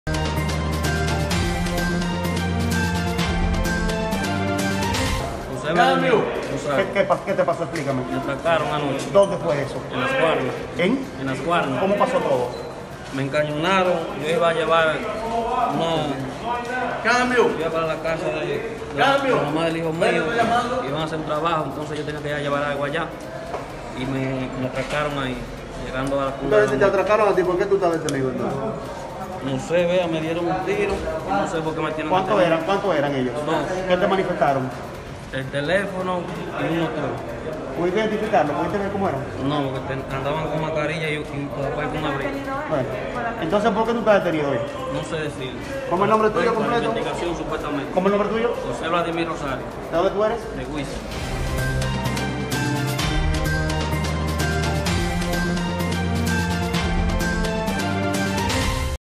Apresan e hieren a joven luego de ser atracado en Las Guaranás
San Francisco de Macorís. - Un joven fue detenido por miembros de la policía luego de que supuestamente fuese atracado y herido de bala en el municipio de Las Guaranás.

El Joven a quien se le identificó como José Bladimir Rosario, residente en la comunidad de Guiza, dijo que el mismo fue atracado por dos desconocidos, que lo despojaron de su teléfono celular cuando se disponía a llevar algo para un trabajo al municipio de Las Guaranás.

Al preguntarle al joven porque se encontraba detenido dijo que lo tenían preso y herido por falta de identificación.